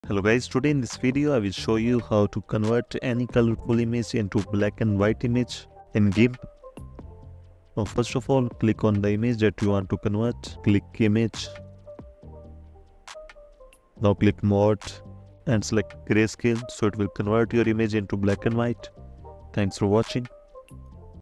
How To Convert Colorful Image To Black And White In GIMP - Full Guide
You will learn how to convert any image to black and white image in GIMP